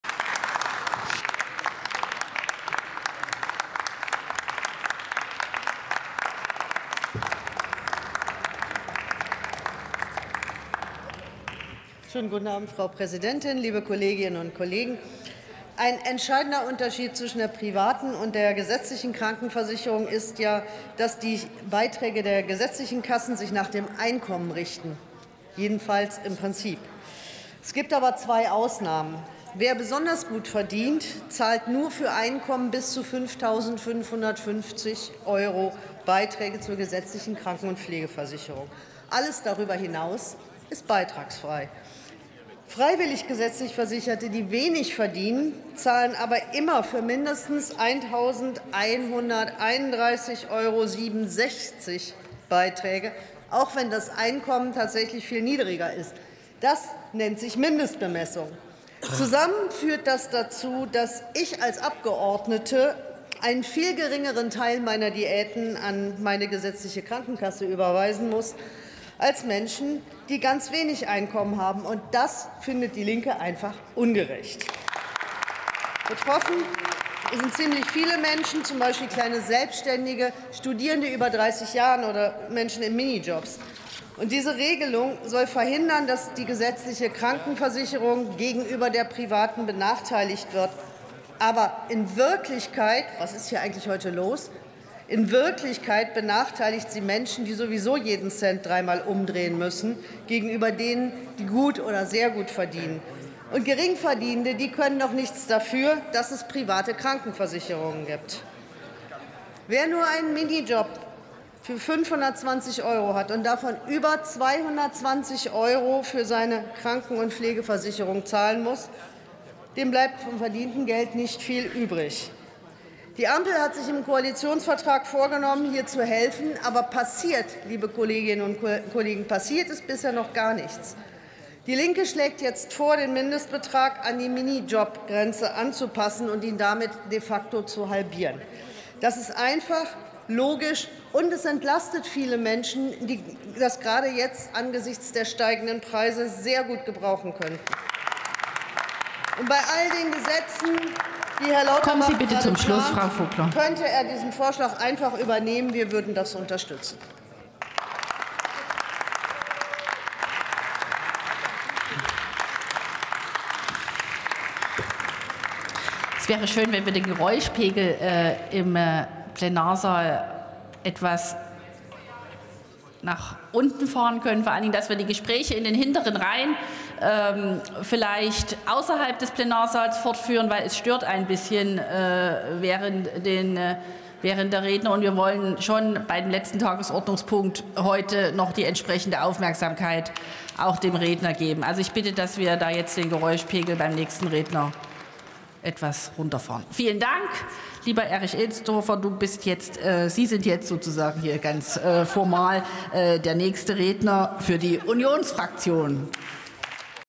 Absenkung der Mindestbemessung in der gesetzlichen Krankenversicherun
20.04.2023 - Kathrin Vogler: Die Mindestbemessung für freiwillig Versicherte in der gesetzlichen Krankenversicherung belastet gerade Menschen mit sehr geringen Einkommen überproportional. Sie muss abgesenkt werden, um eine der schlimmsten Ungerechtigkeiten in der Krankenversicherung abzumildern.